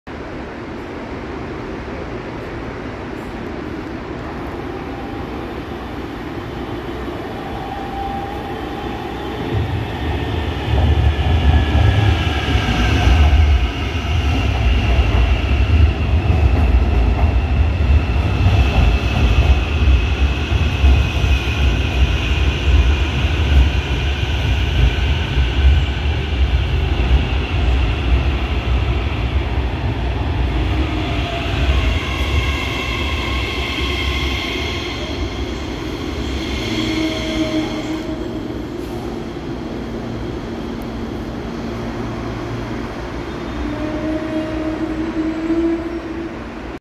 Série 2500 (U504) chegando na estação Aeroporto Guarulhos
U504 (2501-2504) na estação terminal